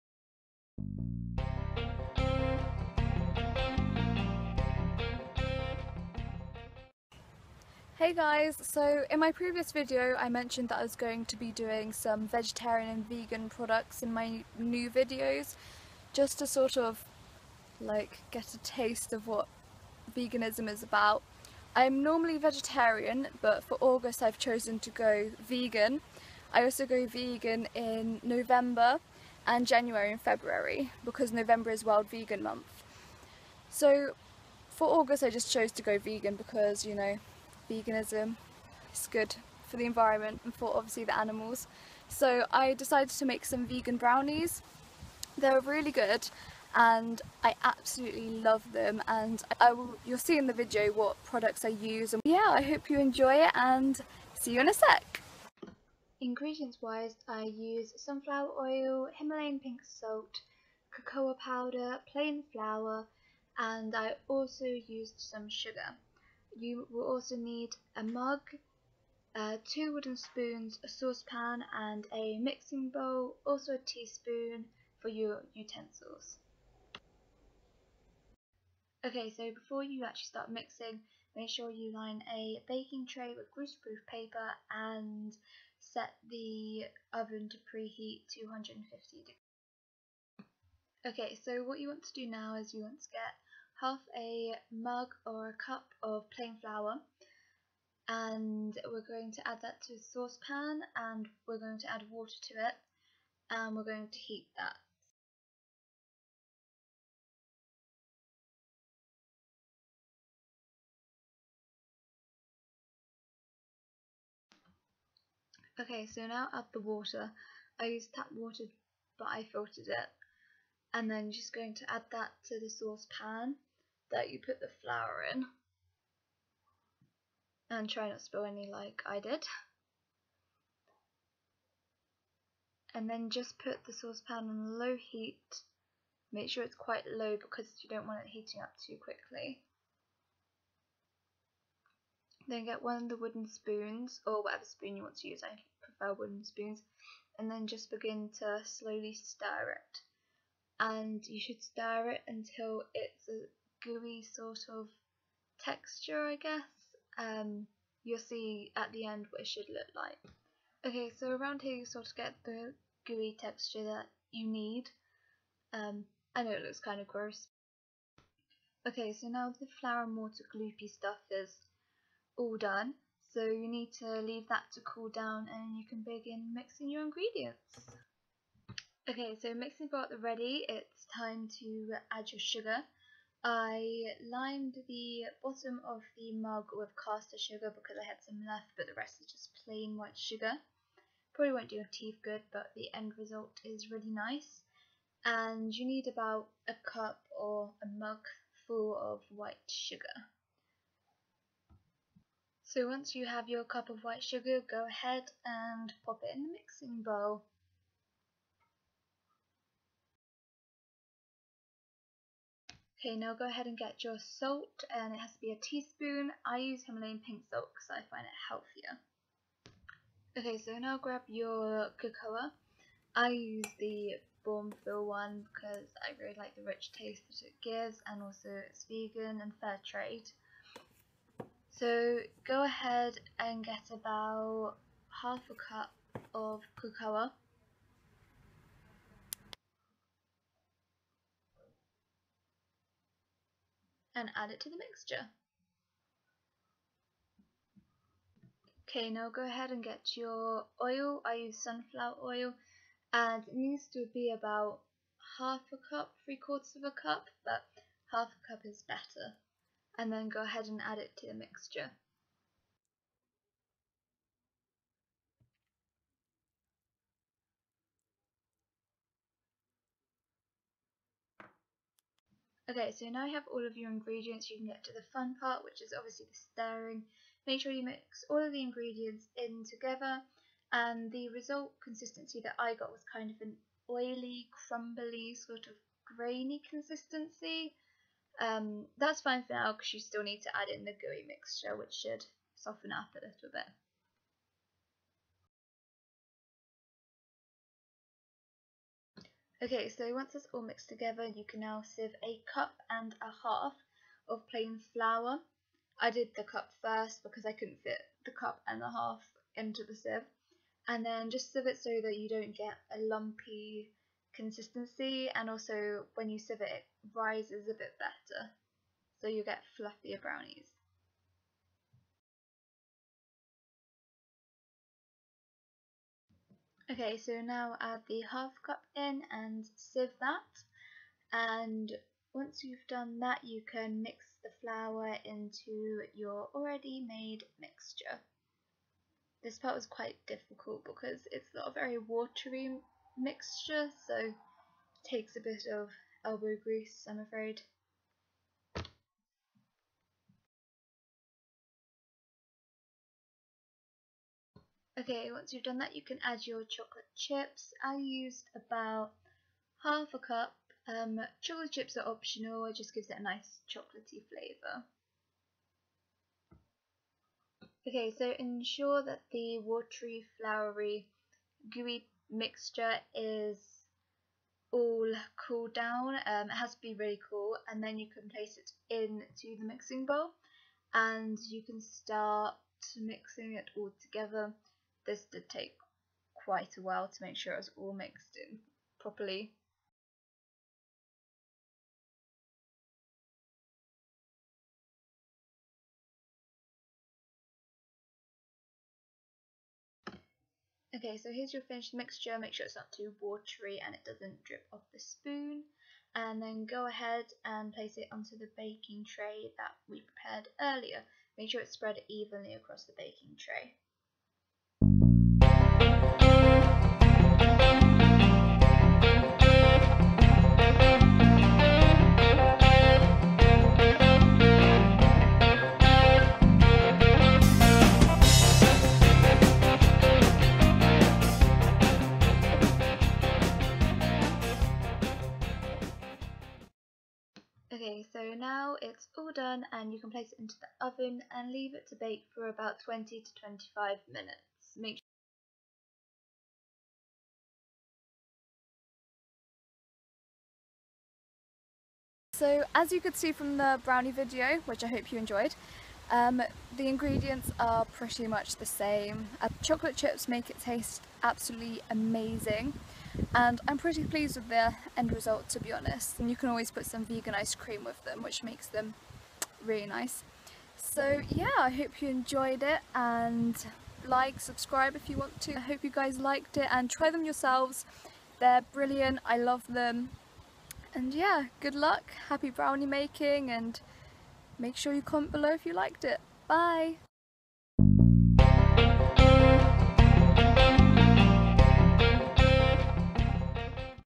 Chocolate VEGAN Brownies
Hey,

So this is my first proper video of *making something*. These brownies are so yummy and you definitely do not need to be vegan to enjoy them.

The recipe is included in the video but just so you know you will need:

Salt (I used Himalayan pink salt but normal salt can be used)
Sugar
Plain flour
Cocoa Powder
Water
Sunflower oil
Chocolate Chips

Utensils:

2 x Wooden spoons
1 Saucepan
1 Teaspoon
1 Mixing bowl
1-2 Mugs (I used 1 mug but 2 might make it easier for some items)
Let me know how they turn out!